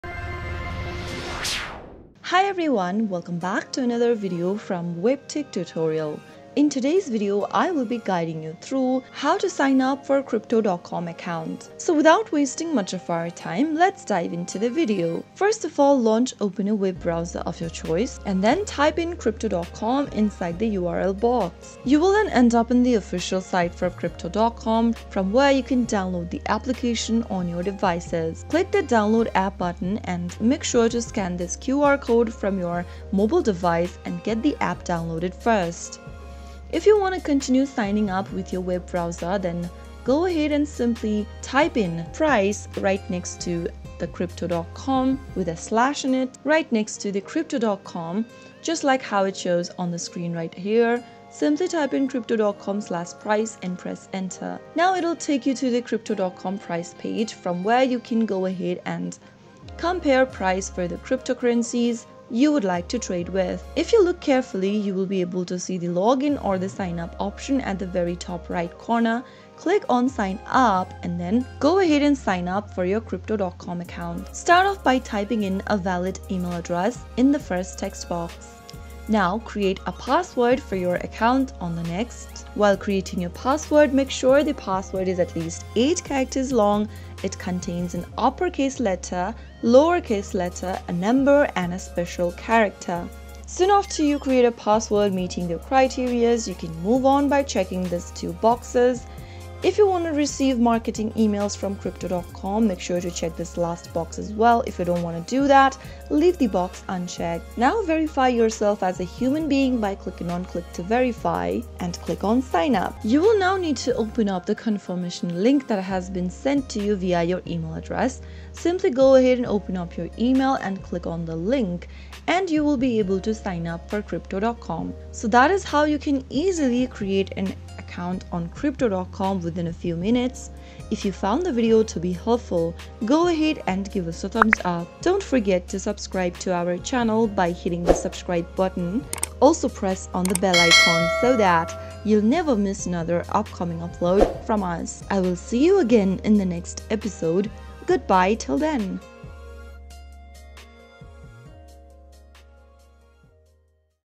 How to Sign Up Crypto.com Account 2024? Create New Crypto.com Account
Embark on your crypto journey with our comprehensive tutorial on how to sign up for Crypto.com. In this video, we'll walk you through the seamless process, ensuring you're ready to explore the exciting world of cryptocurrencies. Join us and get started on your crypto adventure now!